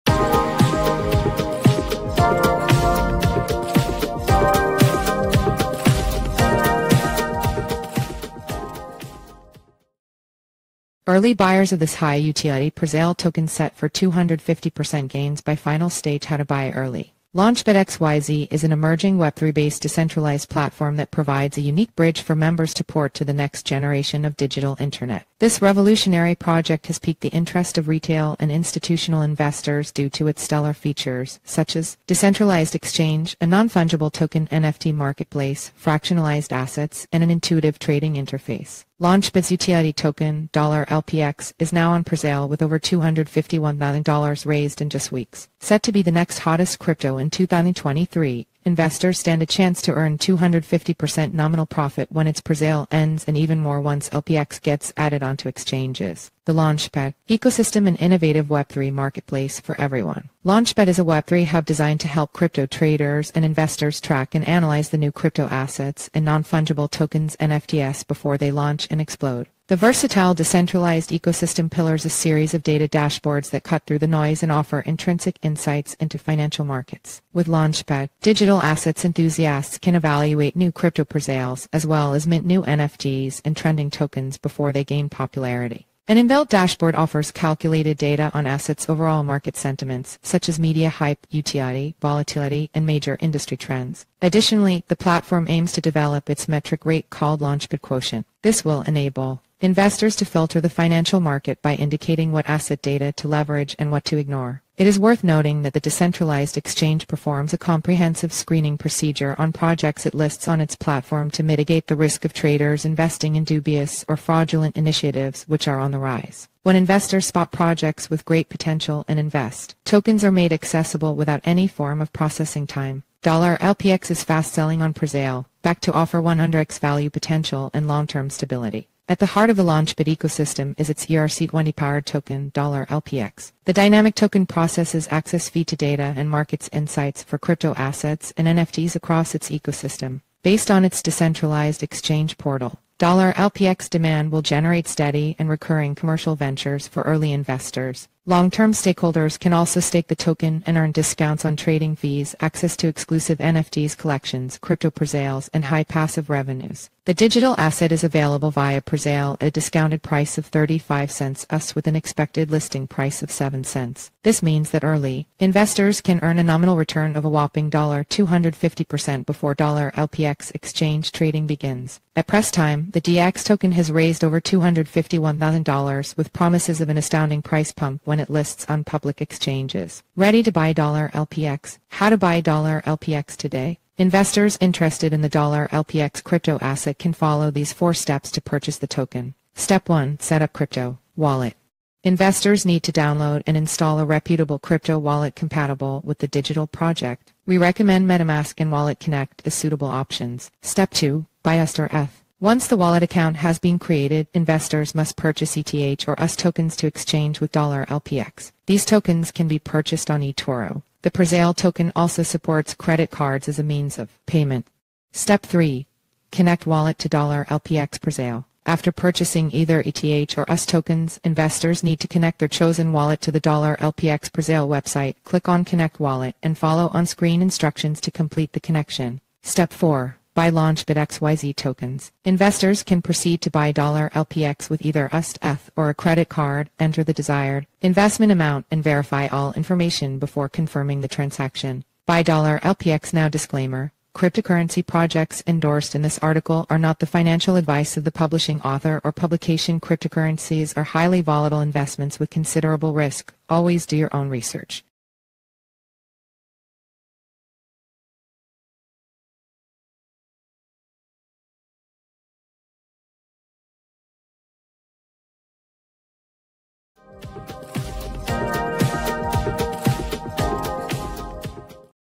Early Buyers of This High Utility Presale Token Set for 250% Gains by Final Stage – How to Buy
Launchpad XYZ is an emerging Web3-based decentralized platform that provides a unique bridge for members to port to the next generation of digital internet.

This revolutionary project has piqued the interest of retail and institutional investors due to its stellar features, such as decentralized exchange, a non-fungible token (NFT) marketplace, fractionalized assets, and an intuitive trading interface.

Launchpad's utility token, $LPX, is now on presale with over $251,000 raised in just weeks.

Set to be the next hottest crypto in 2023, investors stand a chance to earn 250% nominal profit when its presale ends and even more once LPX gets added onto exchanges.

The Launchpad Ecosystem – An Innovative Web3 Marketplace for Everyone.

Launchpad is a Web3 hub designed to help crypto traders and investors track and analyze the new crypto assets and non-fungible tokens (NFTs) before they launch and explode.

The versatile decentralized ecosystem pillars a series of data dashboards that cut through the noise and offer intrinsic insights into financial markets.

With Launchpad, digital assets enthusiasts can evaluate new crypto presales, as well as mint new NFTs and trending tokens before they gain popularity.

An in-built dashboard offers calculated data on assets' overall market sentiments, such as media hype, utility, volatility, and major industry trends.

Additionally, the platform aims to develop its metric rate called "Launchpad Quotient." This will enable investors to filter the financial market by indicating what asset data to leverage and what to ignore.

It is worth noting that the decentralized exchange performs a comprehensive screening procedure on projects it lists on its platform to mitigate the risk of traders investing in dubious or fraudulent initiatives, which are on the rise.

When investors spot projects with great potential and invest, tokens are made accessible without any form of processing time.

$LPX Is Fast-Selling on Presale: Backed to Offer 100x Value Potential and Long-Term Stability.

At the heart of the Launchpad ecosystem is its ERC-20-powered token, $LPX. The dynamic token processes access fee to data and markets insights for crypto assets and NFTs across its ecosystem.

Based on its decentralized exchange portal, $LPX demand will generate steady and recurring commercial ventures for early investors.

Long-term stakeholders can also stake the token and earn discounts on trading fees, access to exclusive NFTs collections, crypto presales, and high passive revenues.

The digital asset is available via presale at a discounted price of $0.035 USDT with an expected listing price of $0.07.

This means that early investors can earn a nominal return of a whopping $250% before $LPX exchange trading begins.

At press time, the DEX token has raised over $251,000, with promises of an astounding price pump when it lists on public exchanges.

Ready to buy $LPX?

How to Buy $LPX Today.

Investors interested in the $LPX crypto asset can follow these four steps to purchase the token.

Step 1: Set Up Crypto Wallet.

Investors need to download and install a reputable crypto wallet compatible with the digital project. We recommend Metamask and Wallet Connect as suitable options.

Step 2: Buy USDT or ETH.

Once the wallet account has been created, investors must purchase ETH or USDT tokens to exchange with $LPX.